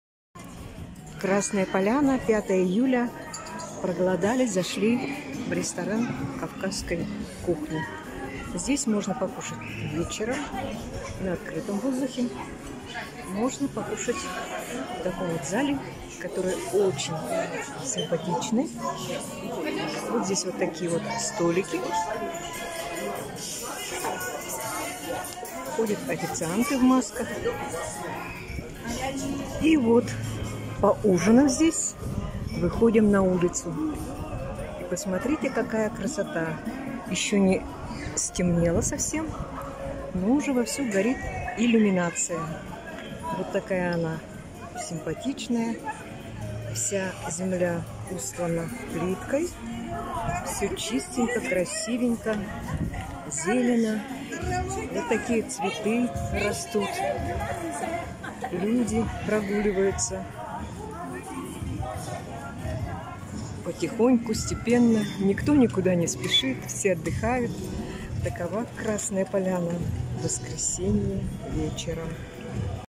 Сколько стоит Покушать?
Красная Поляна уже встречает туристов.
Работают все горнолыжные курорты, канатные дороги, отели, рестораны, экскурсионные бюро.
Гостеприимство Сочи растёт вместе с опустошением наших карманов.
Покушать в ресторане стоит от двух тысяч, ски пасс (подъем в горы на фуникулере полторы тысячи )...
Отдыхай - не хочу!